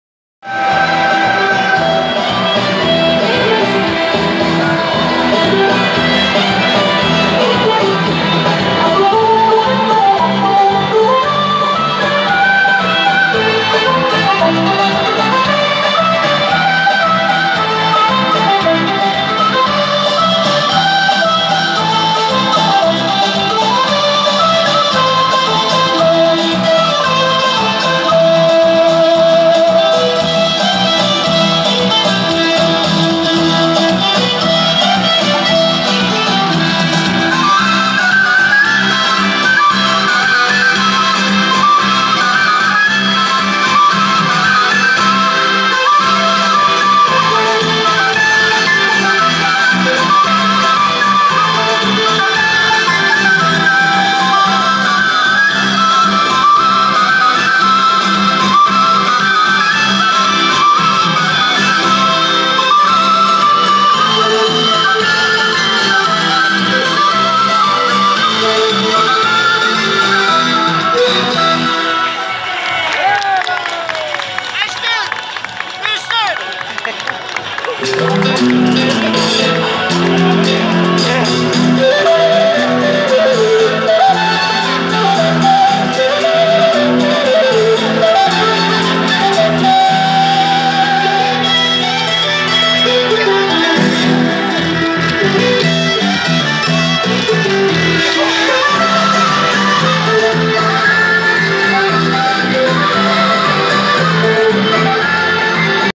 Patate fiesta en honor al señor del terremoto(5)
Año tras año se vive esta fiesta con fe